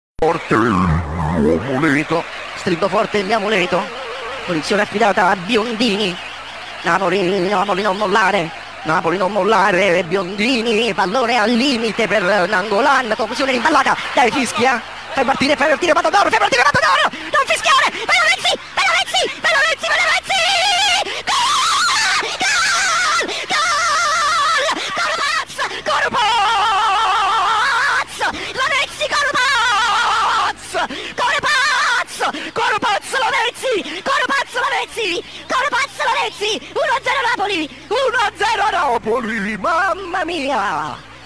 Napoli non mollare zio borgo.
Punizione affidata a Biondini.

Video preso dalla pagina Facebook "Quanto cazxo é forte mario Rui?"